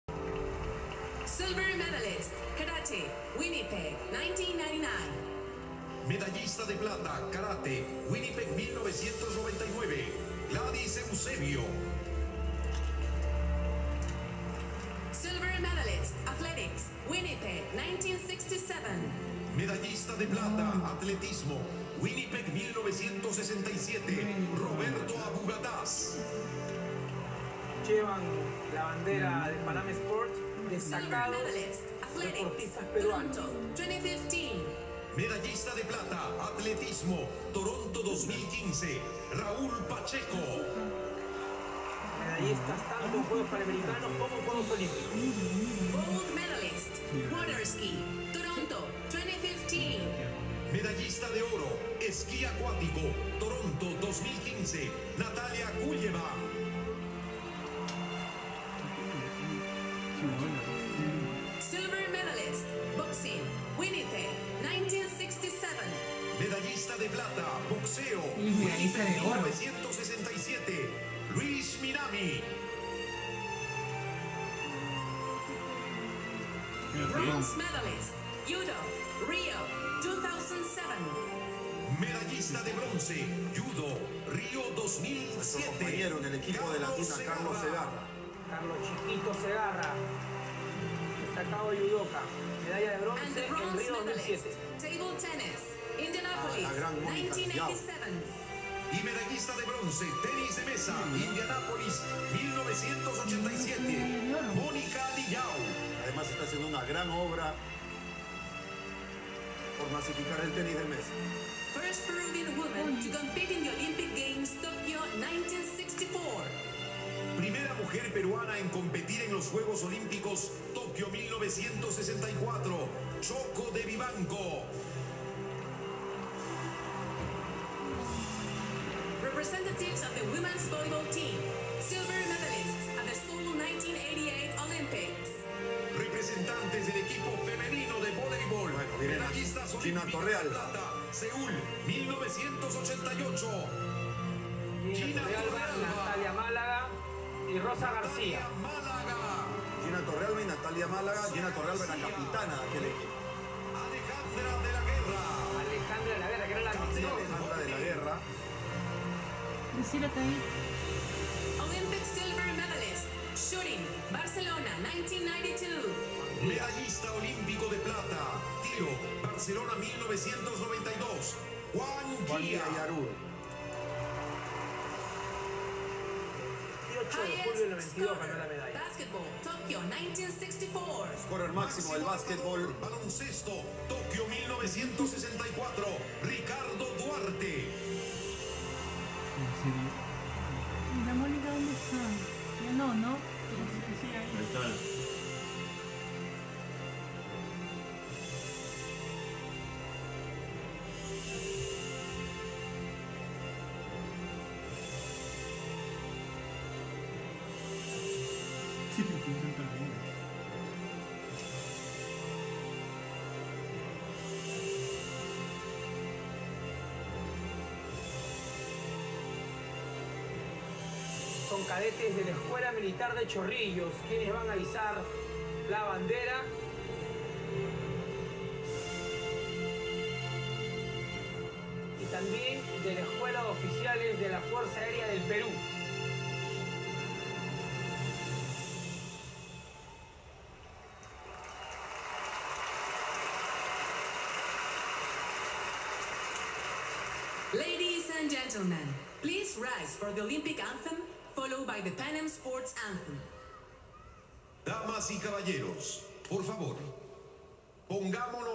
JUEGOS PANAMERICANOS LIMA 2019
Nuestra fundadora del Programa ImpactandoVidas, Mónica Liyau tuvo el honor de cargar la bandera Panamericana junto al lado de otras personalidades del deporte peruano el día de la inauguración de los Juegos Panamericanos Lima 2019 en el Estadio Nacional.